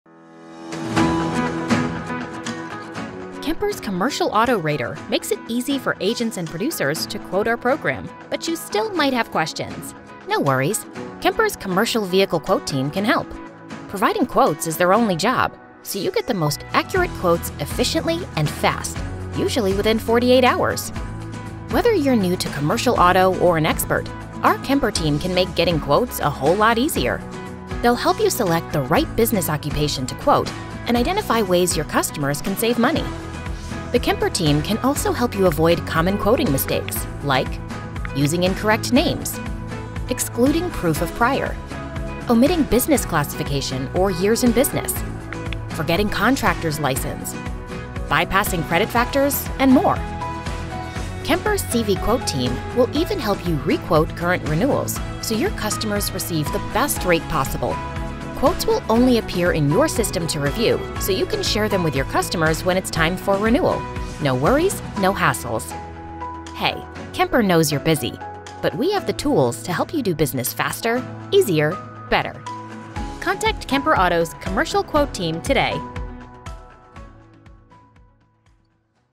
Kemper - Video 10 | Explainer Video by Animation Explainers
Here is the tenth video in the series that we worked on with Kemper in the Insurance industry. To see more examples of great explainer videos see AnimationExplainers.com.